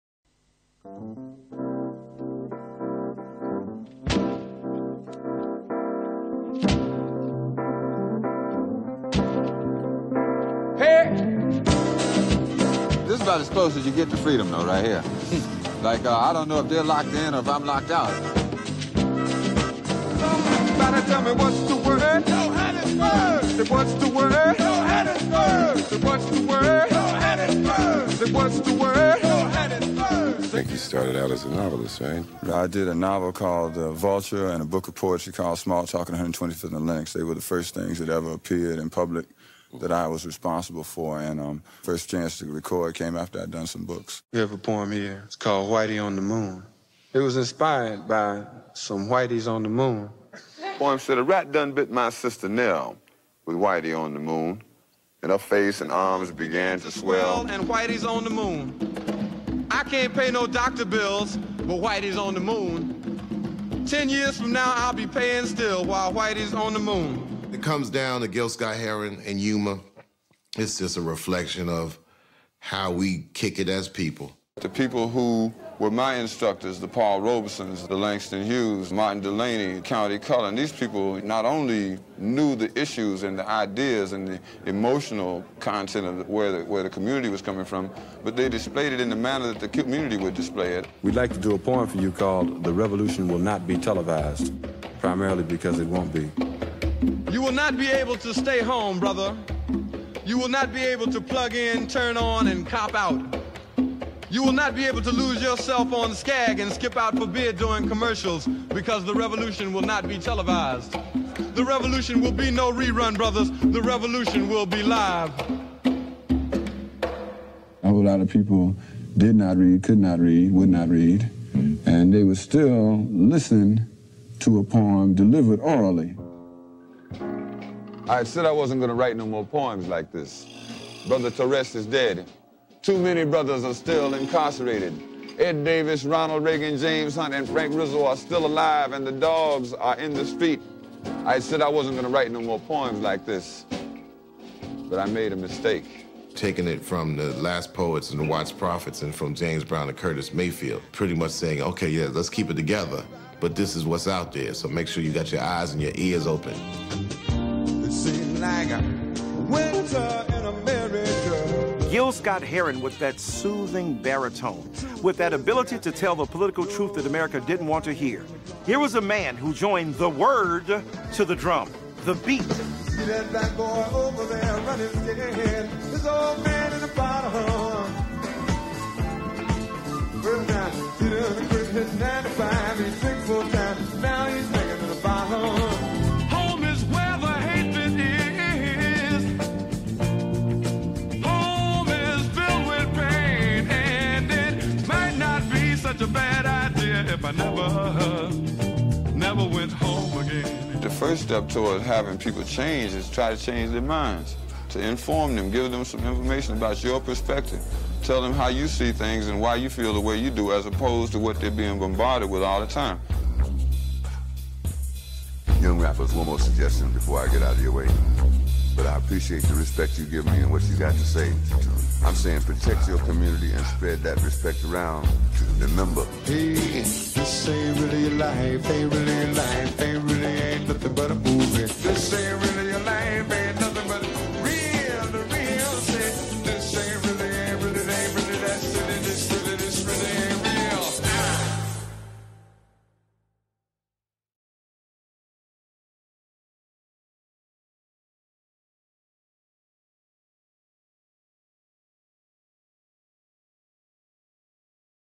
Gil Scott-Heron Spotlight | 2021 Induction Ceremony
In-Arena video feature spotlighting Inductee Gil Scott-Heron, shown during the 2021 Induction Ceremony.
RockHall2021 Induction Ceremony
October 30, 2021 | Cleveland, OH

Sign up for our newsletter to get the latest info delivered straight to your inbox